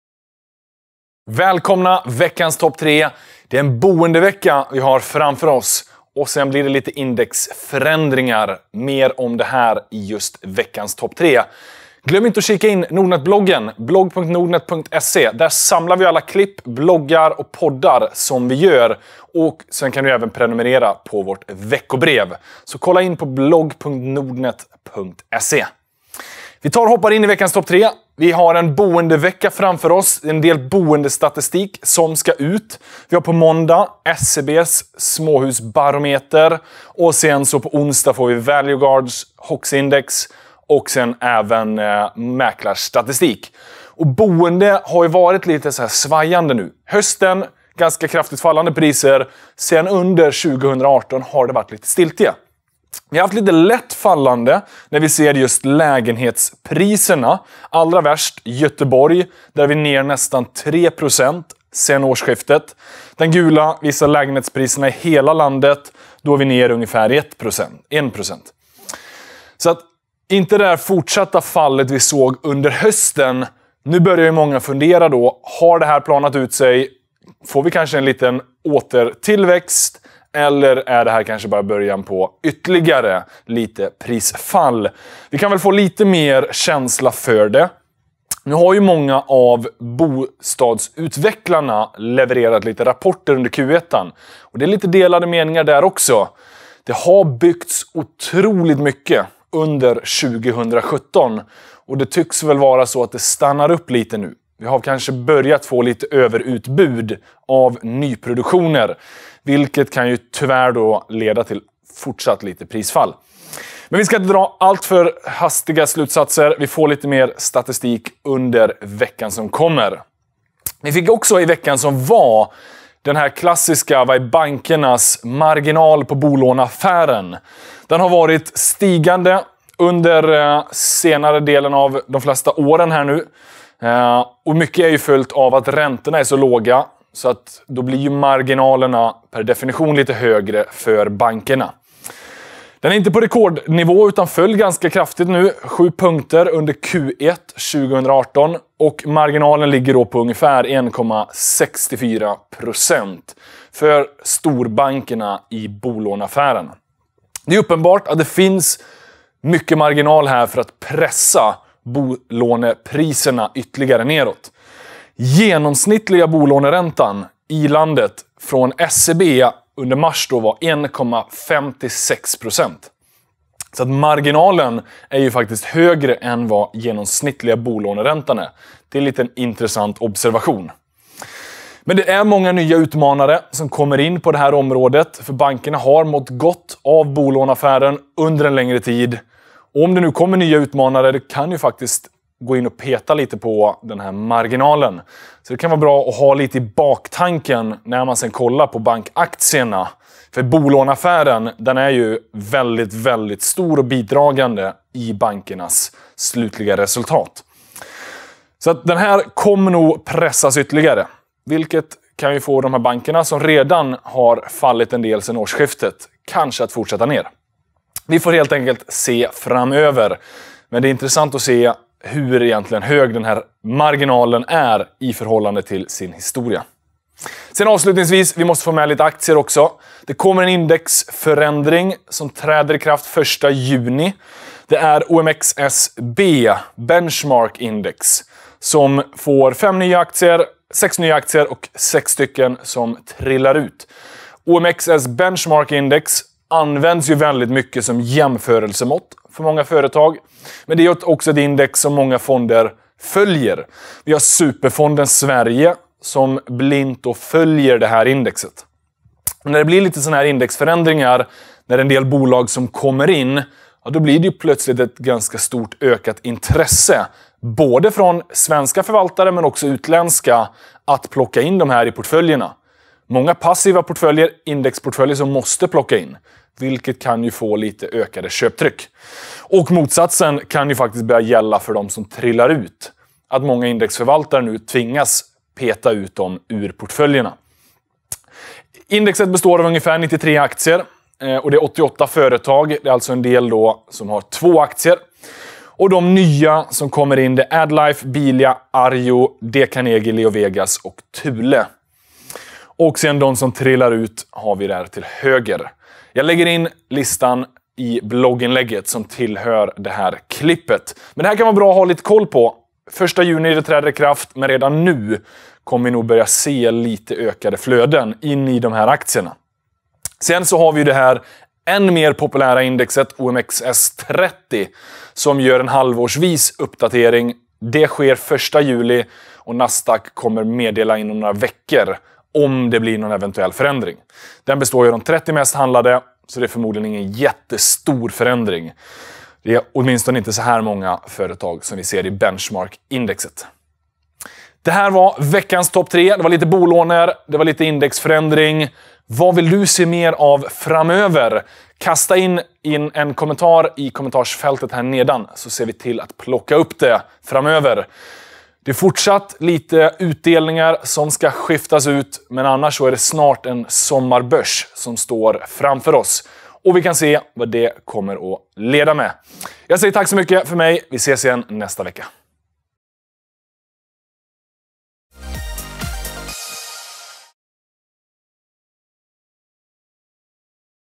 Indexrally - Veckans Topp 3 [V.20 2018]
Mycket bostadsstatistik släpps i veckan, detta plockar vi upp. Sen blir det sedvanlig förändring i index vilket kan skapa ökat intresse för en del bolag, samtidigt som ett gäng blir bänkade. Mer om detta i veckans topp 3.

Utdelning och Rapporter:
Det är inte helt över på rapportfronten ännu, men klart svalare än tidigare. Här kommer veckans rapporter:

Mån: Boule Diagnostics (kl 15.00), Arctic Paper
Tis: SSM (kl 7.30), Actic (kl 7.45), Hoist (kl 8.00), Precise (kl 8.00), Vostok New Ventures (kl 8.00), Addtech (kl 8.15), Kabe (kl 16.00), Opus (före börsöppning)
Ons: Wilson Therapeutics (kl 7.00), Bergman & Beving (kl 7.45), BTS (kl 8.00), Eastnine (kl 8.00), Humana (kl 8.00), Nibe (kl 8.00), Wise Group (kl 8.30), Bong, Empir, Nilörn
Tors: Ambea (kl 7.00), Björn Borg (kl 7.30), Balco (kl 8.00), Boozt (kl 8.00), Oncopeptides (kl 8.00), Active Biotech (ändrat datum fr 26/4) (kl 8.30), Sensys Gatso (kl 8.30), Endomines (kl 8.45)
Fre: Besqab (kl 7.30), Astra Zeneca (kl 8.00)
Även några bolag som handlas exklusive utdelning i veckans som kommer:

Tis: Boule Diagnostics (2:00 kr)
Ons: Actic (0:50 kr), B3IT (2:17 kr), Bahnhof (0:50 kr), Billerud Korsnäs (4:30 kr), Feelgood (0:08 kr), FM Mattsson (3:00 kr), Generic (1:00 kr + 0:70 kr extra), Kabe (5:50 kr), Kindred (0:551 gbp), Micro Systemation (2:20 kr), Precio Fishbone (1:20 kr), Zinzino (0:45 kr)
Tors: Alimak (2:30 kr), Byggpartner (2:50 kr), Empir (0:81 kr), Genova Property (2:63 kr i preferensaktie), Hoist (1:90 kr), Humana (0:60 kr), New Wave (1:70 kr), Nibe (1:05 kr), Nilörn (4:00 kr), Rottneros (0:30 kr+extra 0:07 kr), Wise Group (3:00 kr), Volati (0:50 kr i stamaktie)
Fre: Avensia (0:18 kr), Balco (1:00 kr), Drillcon (0:33 kr), Munters (0:30 kr), Opus (0:05 kr)
I övrigt så är svenska börsen öppen som vanligt hela veckan. Bra att påminnas om när vi haft 2 veckor av lite förkortade handelstider. Oslo börs har däremot stängt på torsdag, 17e maj, för nationalfirande.

💰 Få gratis courtage upp till 80.000kr om du öppnar konto hos Nordnet idag